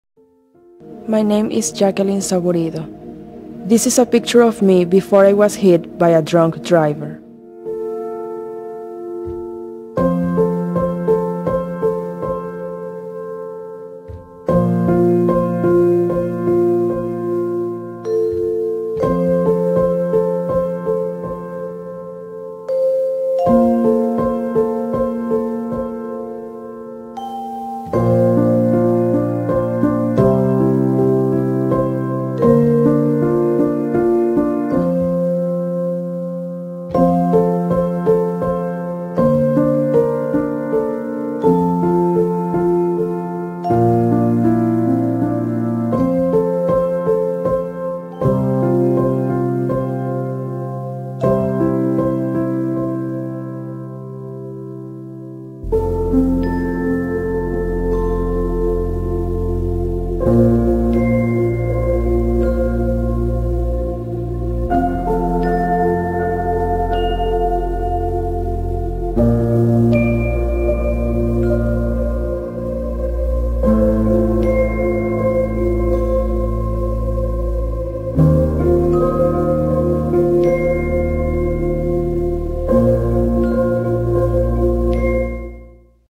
Jacqui Saburido, the face of campaigns against drunken driving, dies at 40
Jacqueline Saburido, who became the face of campaigns in Texas and beyond against drunken driving after she was severely burned in a wreck near Lake Travis 20 years ago, died Saturday of cancer in Guatemala, according to her family.